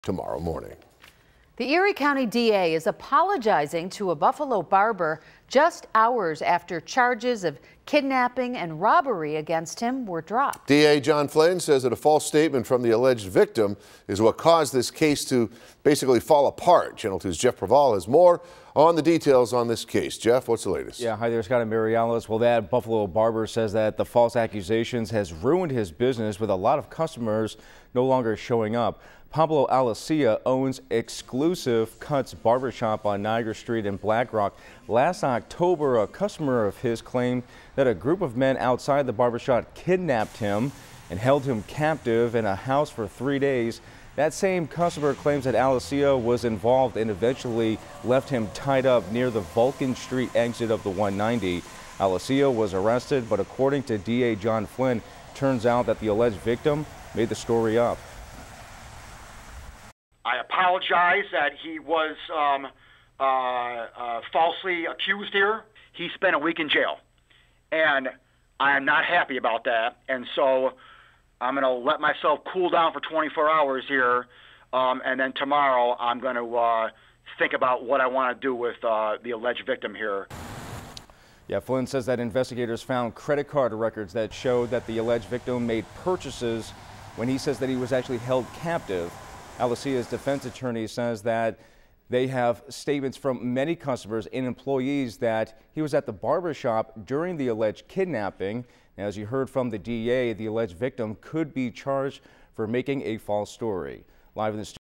DA Flynn Apologizes To Barber Over Arrest
False story about a kidnapping at the barber shop lands a barber in jail of a week